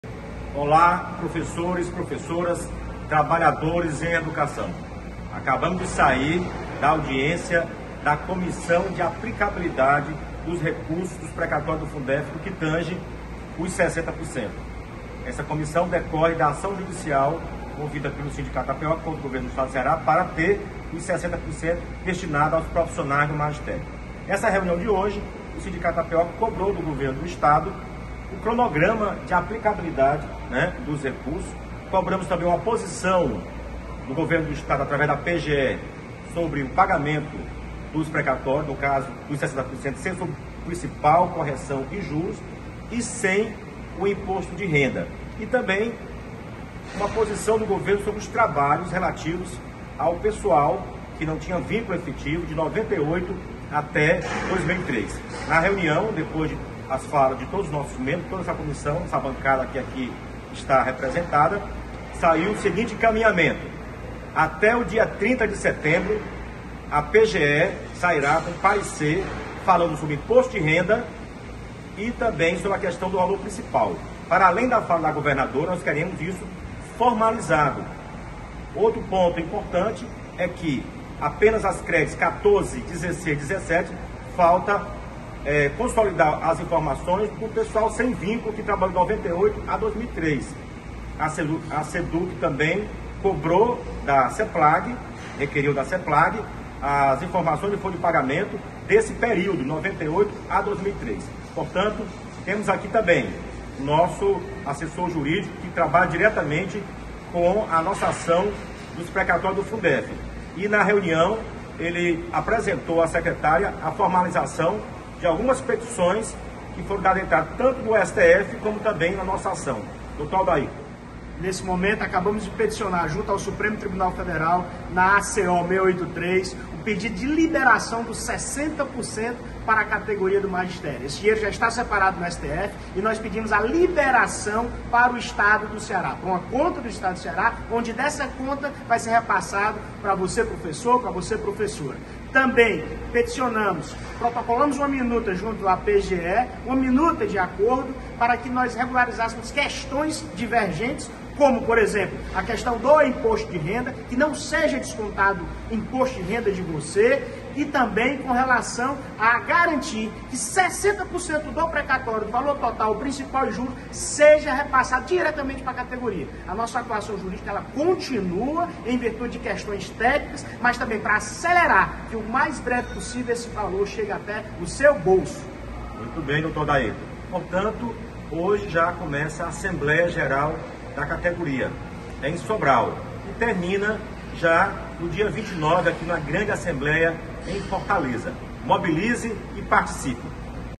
APEOC COBRA RESPOSTAS EM REUNIÃO E PGE APRESENTARÁ PARECER ATÉ 30/09
A Comissão de Aplicabilidade dos 60% do Precatório do FUNDEF da rede estadual, que decorre da ação judicial do Sindicato APEOC, reuniu-se, nesta terça-feira (20), para discutir questões pendentes.

Representada pelo presidente, professor Reginaldo Pinheiro, a APEOC cobrou o cronograma de pagamento, como também uma posição oficial do governo do Ceará, por meio da PGE, quanto ao pagamento dos 60% sobre o valor integral (principal + juros + correção monetária) e sem o desconto do Imposto de Renda.

A Banca de Advogados Especializada, contratada pela categoria, representada pelo Dr. Aldairton Carvalho, informou na ocasião que também peticionou, junto ao STF, o pedido de liberação dos 60% do Precatório do FUNDEF da rede estadual aos profissionais do Magistério.

ENCAMINHAMENTOS

• Até o dia 30 de setembro, a PGE apresentará um parecer sobre o desconto do IR e pagamento do valor integral;

• Mesa técnica já dispõe da coleta de dados de 1998 a 2003 dos professores temporários, faltam apenas os dados das CREDEs 14, 16 e 17 para que haja a consolidação das informações sobre os(as) professores(as) temporários que trabalharam de 1998 a 2003;

• A SEDUC solicitou da SEPLAG as informações da folha de pagamento de 1998 a 2003;

• Reunião marcada para o dia 23/09, na SEDUC, com o Grupo Técnico, com a participação da SEFAZ no GT;

Participaram da reunião, pela APEOC: a professora Penha Alencar (secretária de assuntos financeiros); o professor Helano Maia (secretário-geral); o professor Maurício Manoel (secretário de assuntos educacionais); e André Carvalho (assessor financeiro). Pelo Banca de Advogados: Dr. Vanderlei Marques. Pela SEDUC: a secretária Eliana Estrela, Marta Emília (coordenadora COGEP); e assessoria. Pela PGE: Dr. João Renato (Procurador-geral Executivo de Contencioso Geral e Administrativo).